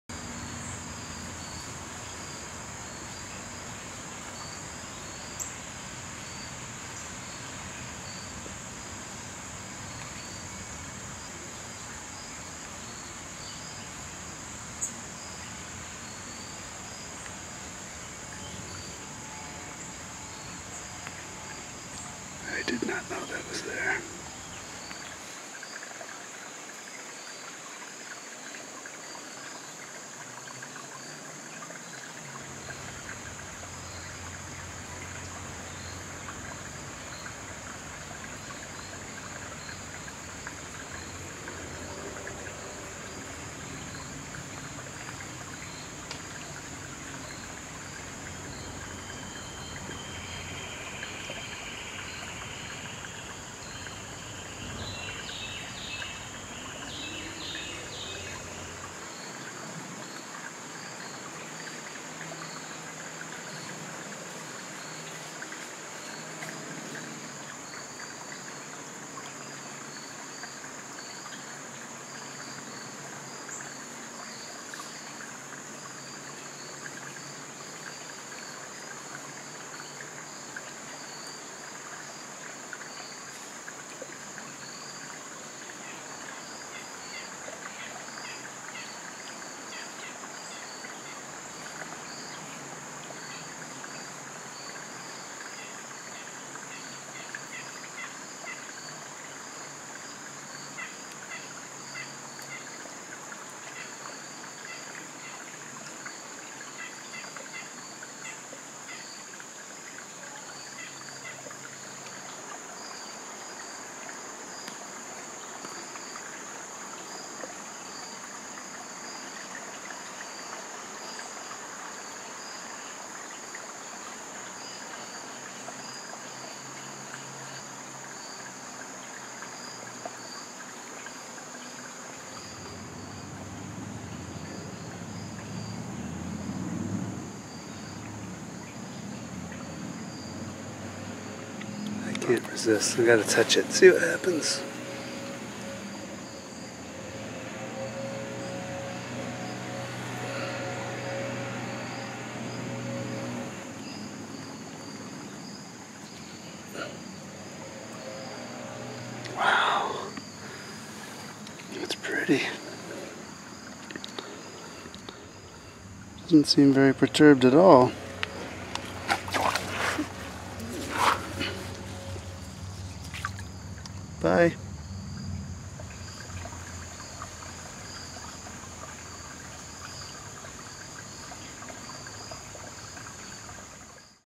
Water Snake & Water Striders in Fall
Nerodia sipedon sipedon on October 14, 2021 in Maynard, Massachusetts.